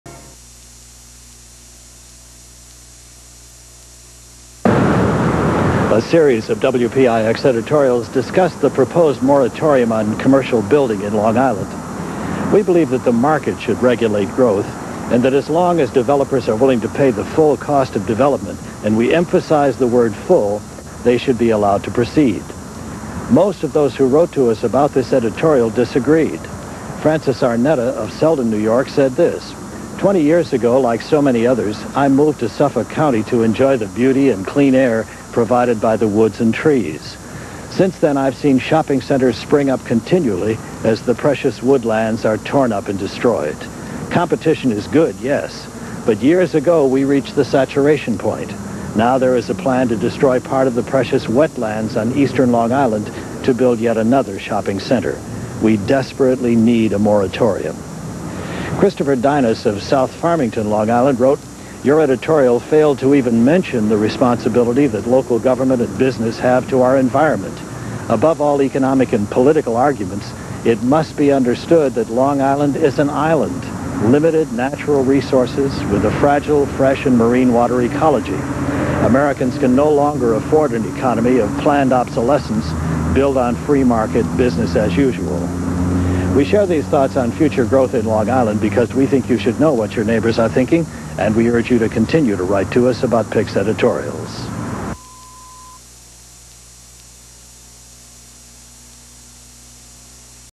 Richard N. Hughes - Editorial Feedback - Growth in Long Island - 1991
The Legendary VP in charge of News in one of the Rare times he does NOT say his famous "What's your Opinion, We'd like to Know" catchphrase...Why? Because this time around, Mr. Hughes is sharing the opinions of viewers on an previous editorial on Growth in Long Island. (Sorry for the Spotty Video).